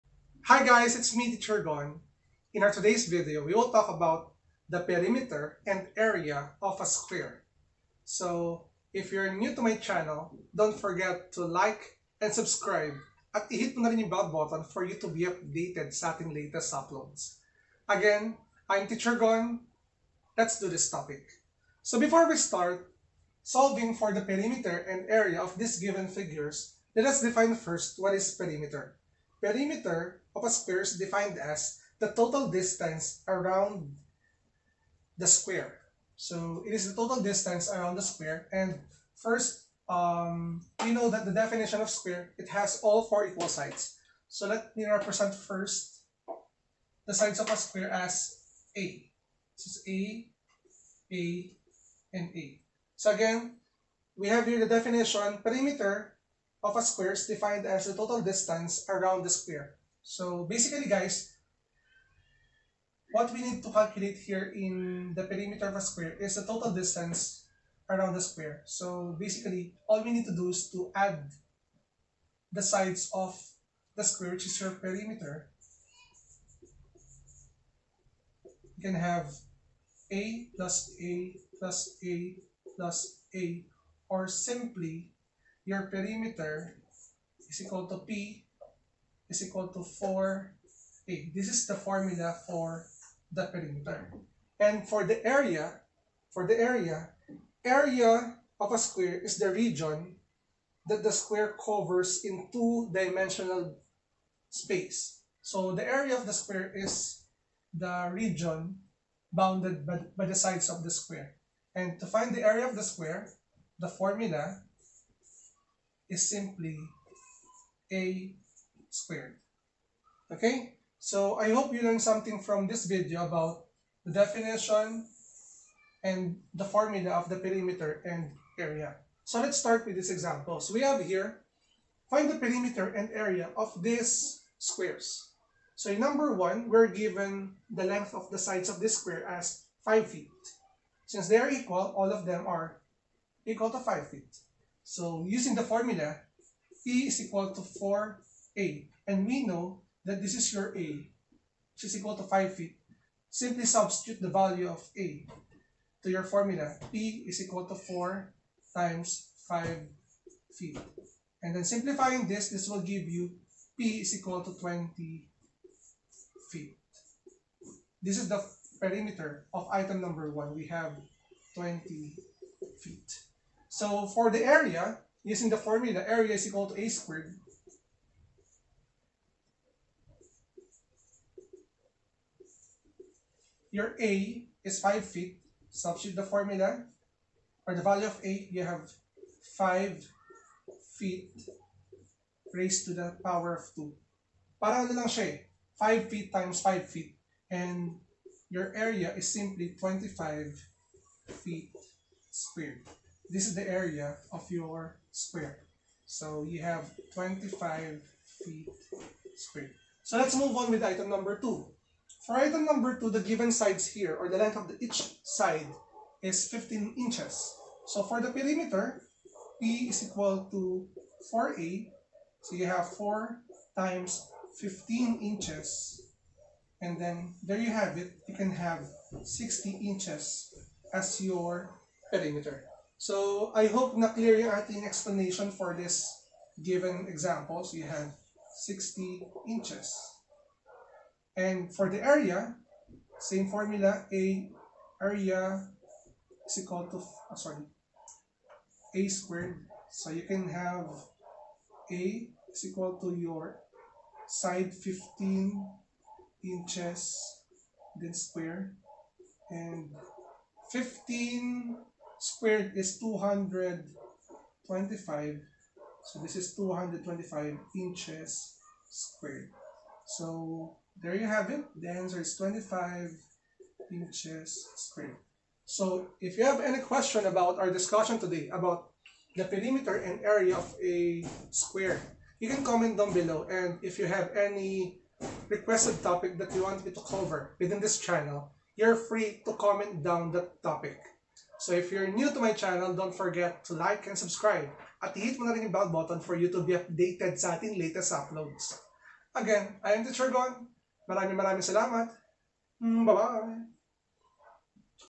Perimeter and Area of SQUARE @MathTeacherGon Civil Service Exam Reviewer
@MathTeacherGon will demonstrate how to solve for the perimeter and area of a square.